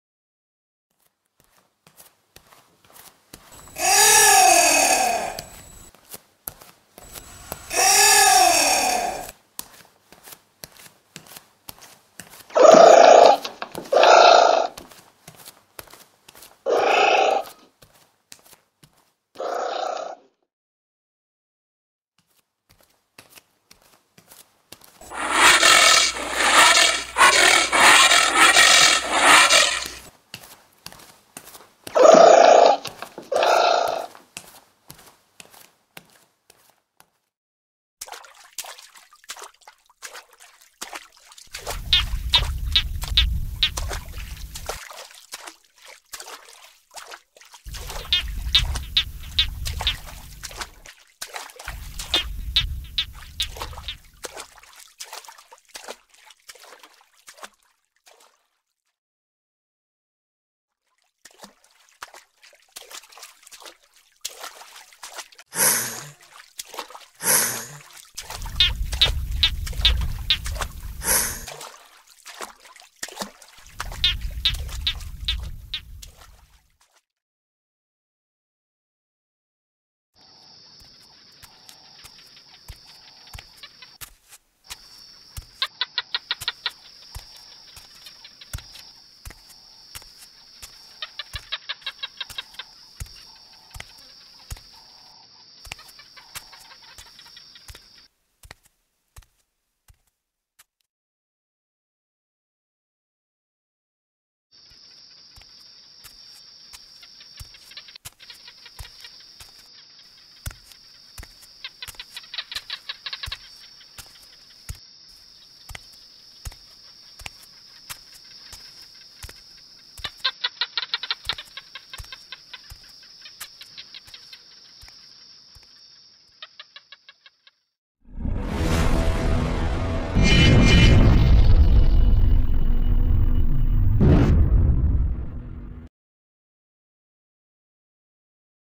SPINY DINOSAUR video green screen. Footage animation of the GREEN SCREEN.
Footage of the SPINY DINOSAUR video on a green screen for video editing. Chromakey animation DINOSAURS .

DINOSAURS ON THE CHANNEL

MORE ANIMALS

INSECTS

 MONSTERS